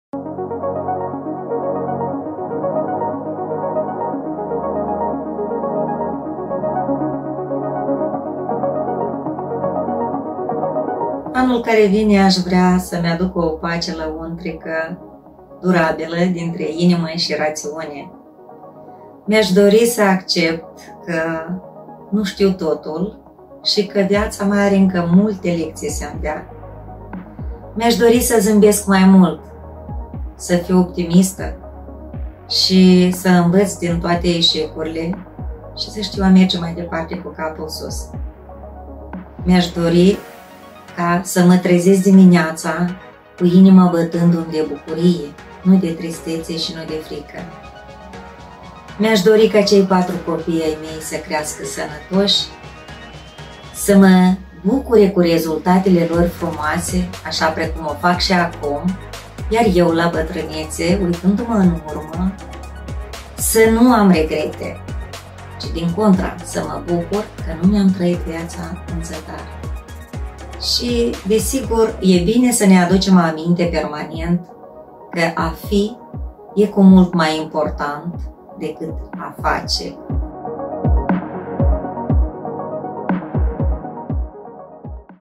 Liliana Cazac în proiectul CEvreaEA (46 de ani)
Liliana Cazac este cunoscută de toți și, probabil, de fiecare în ipostaze diferite. Cei din domeniul media o cunosc drept președintele consiliului de administrare al Busuioc TV, telespectatorii îi privesc emisiunile la această televiziune, melomanii îi admiră piesele, iar apropiații o cunosc drept o mamă iubitoare a 4 copii. Noi am provocat-o să ne spune ce așteptări are de la anul ce vine în proiectul celor 90 de femei pe EA.md.
Da-i click! Share! Comenteaza!